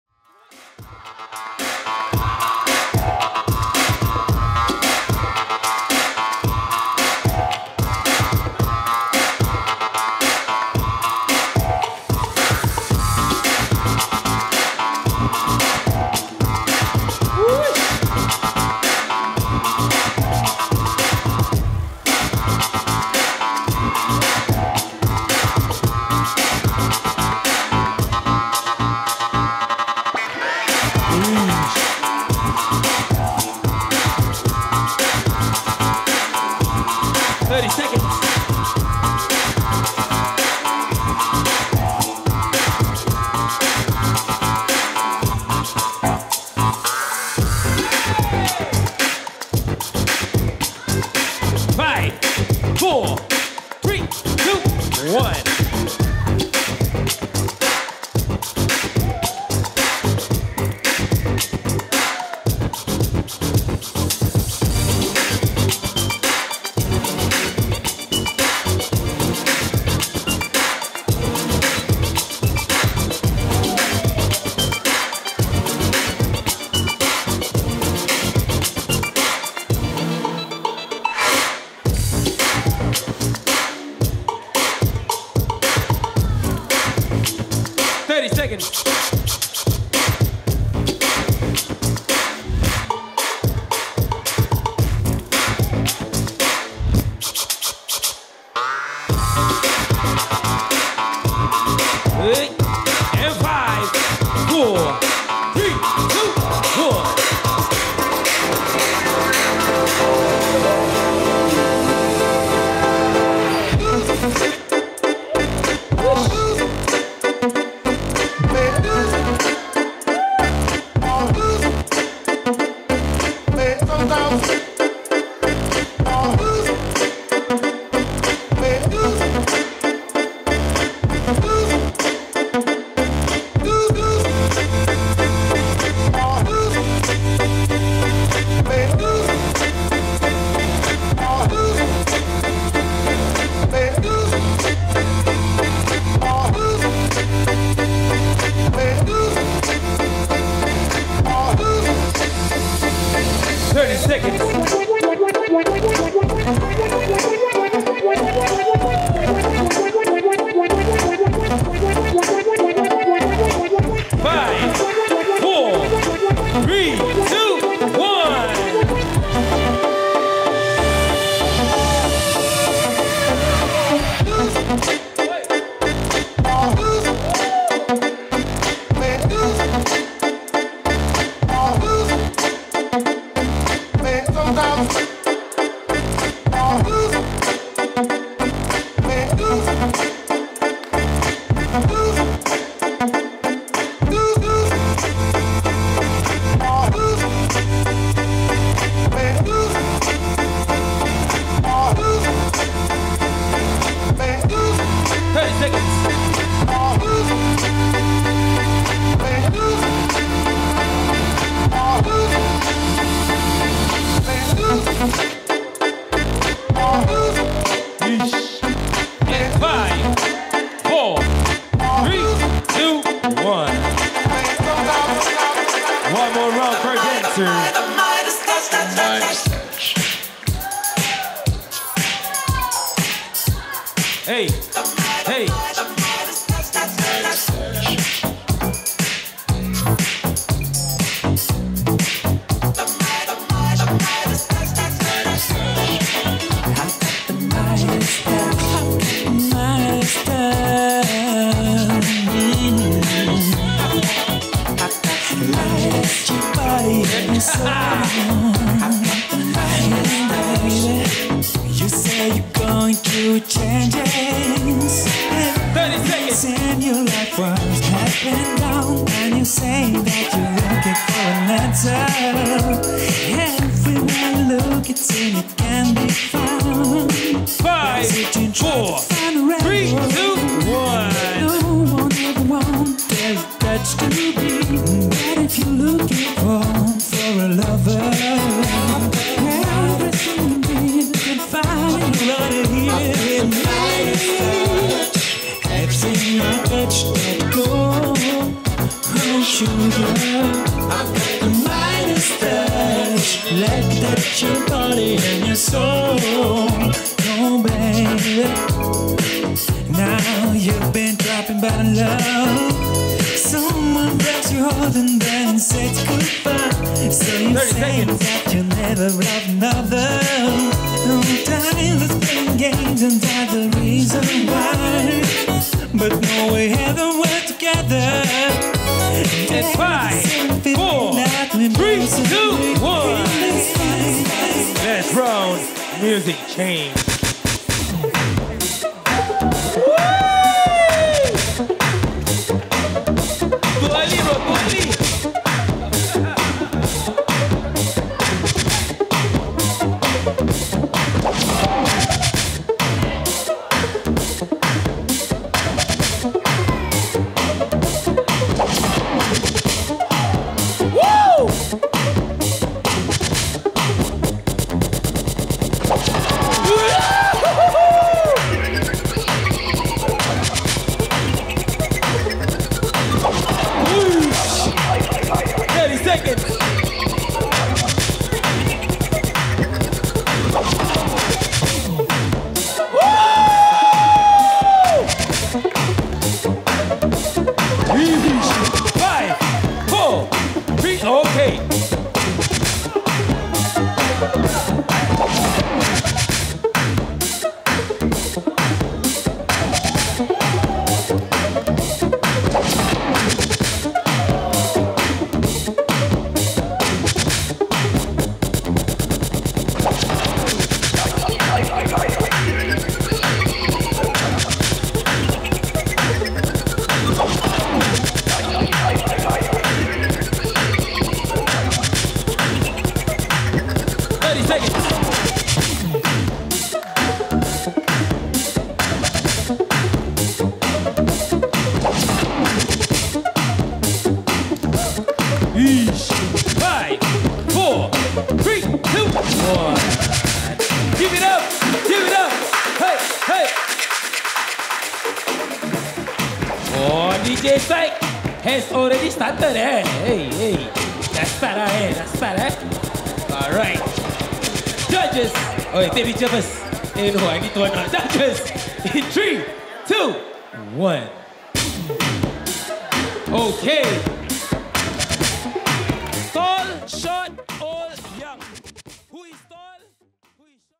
Tall Short Old Young vs Ryu & Yeorin | Popping Top 4 | WDC Singapore 2024 | RPProds
Organized by @DanzPeople

Singapore takes centre stage for the first time as it hosts the first edition of World Dance Colosseum (WDC) Singapore. WDC Singapore features intense 2v2 showdowns in Hip Hop, House, Popping, Waacking, and 1v1 Under 16 Battle. Winners from this electrifying event will be seeded for the prestigious WDC 2024 FINAL in Japan.

Judges:
D Soraki (JPN)
Hiro (JPN)
Marzipan (SG)
Racheezels (SG)
Aaron Tan (SG)

Filmed by TK
Edited by Richard Prayoga
Thumbnail Photo by Jezzie